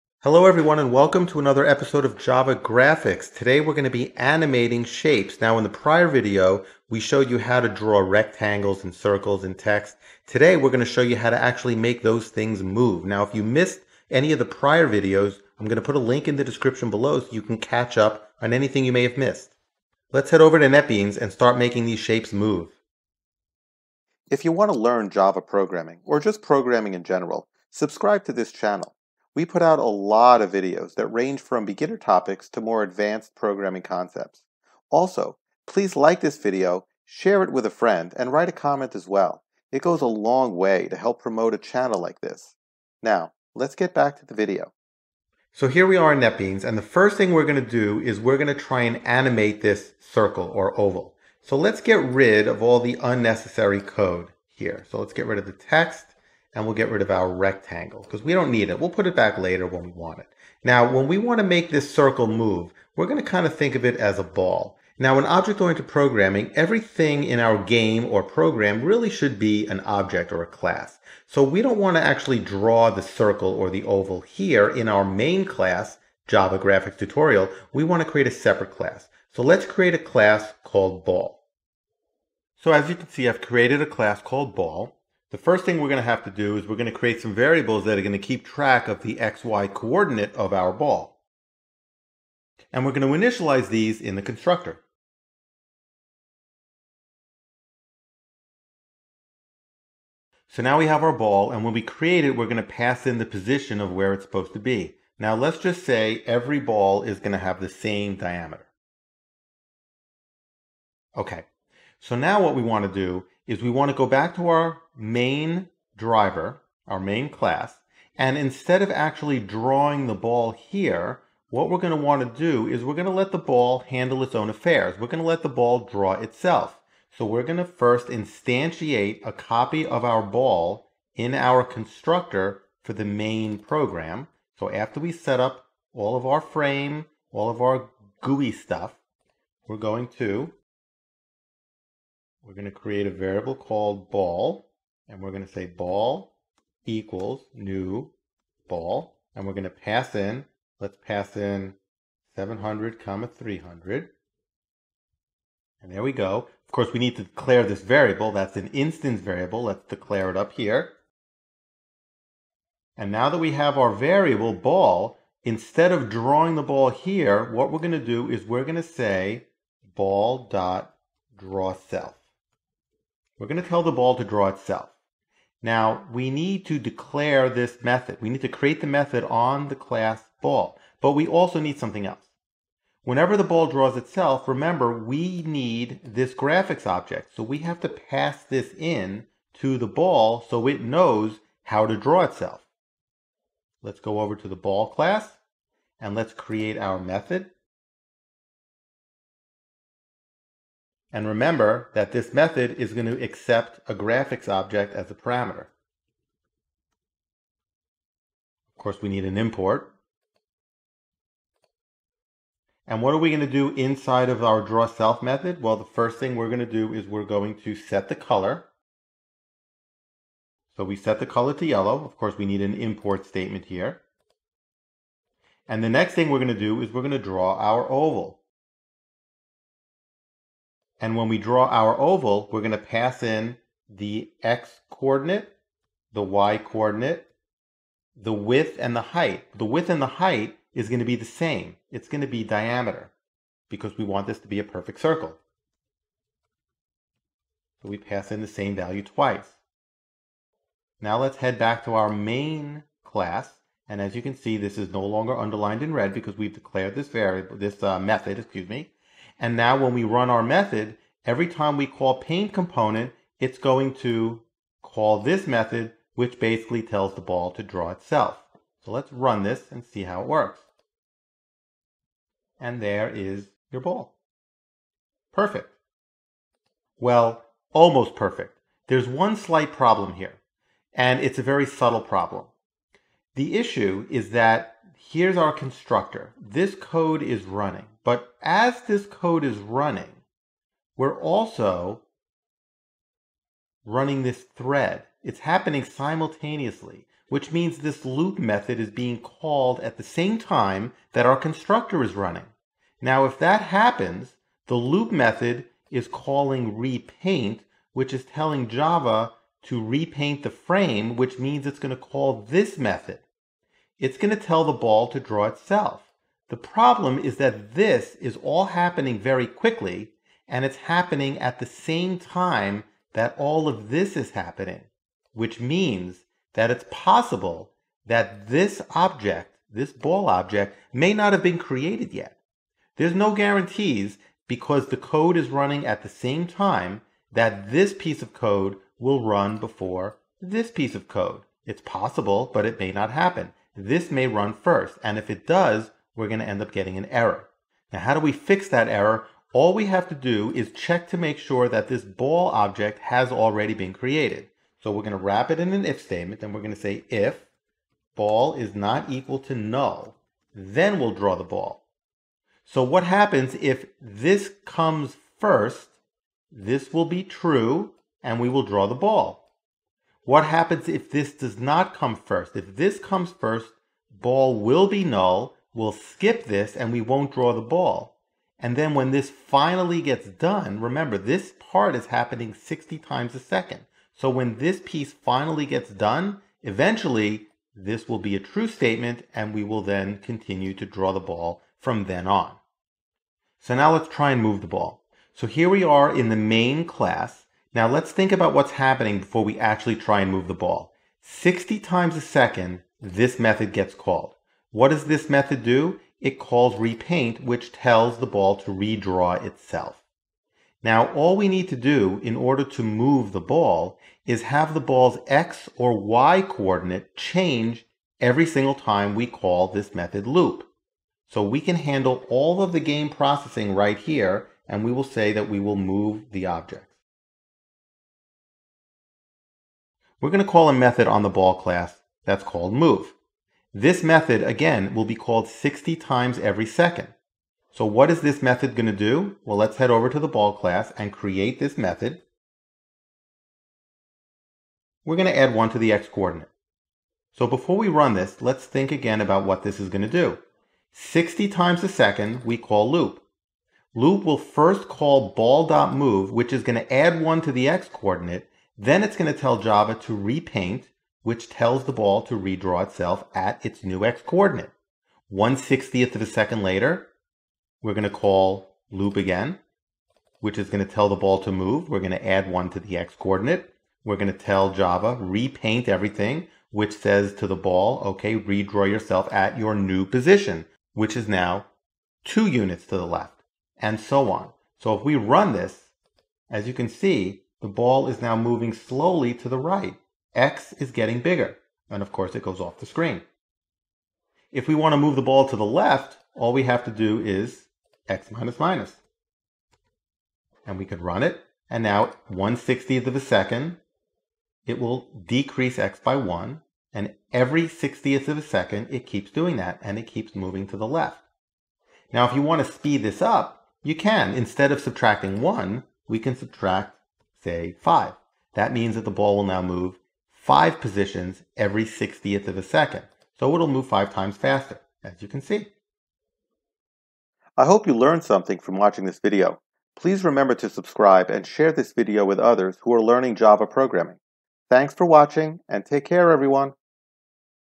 Java Graphics - 03 - Animating Shapes Part 1
This video will teach you how to animate basic shapes in Java.

This is one of a multi-part series on teaching basic concepts of using graphics when programming in Java. I am a computer science and mathematics teacher at both the university and high school level. This series is designed around that curriculum. I hope these videos help you.